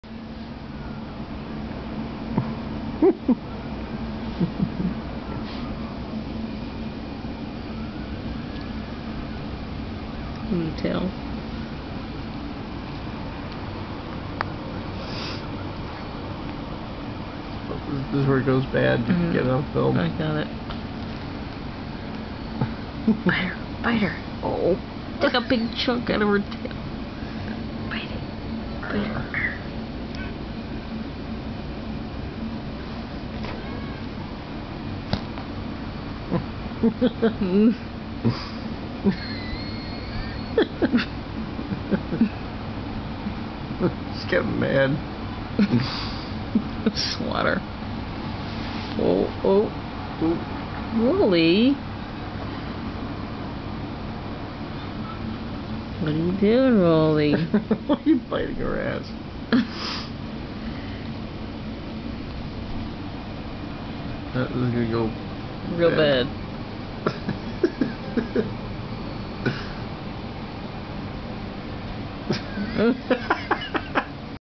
Siamese Cats
RIP Rolly
These were my two cats. their names are Rock and Roll. Rolly is the one biting the other one's butt.